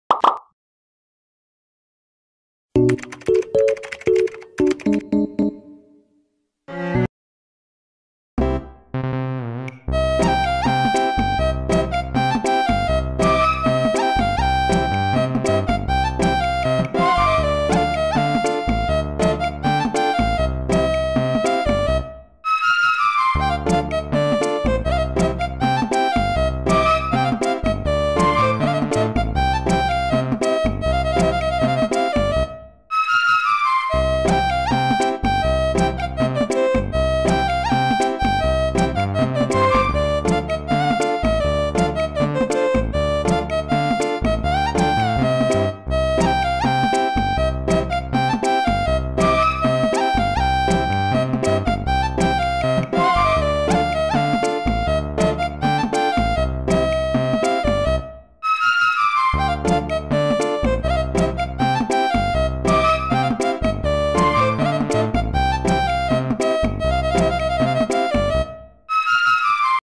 What do I use to make the Evillious characters?
Well now you know the history of the drawn characters in the Evillous series I'm doing lol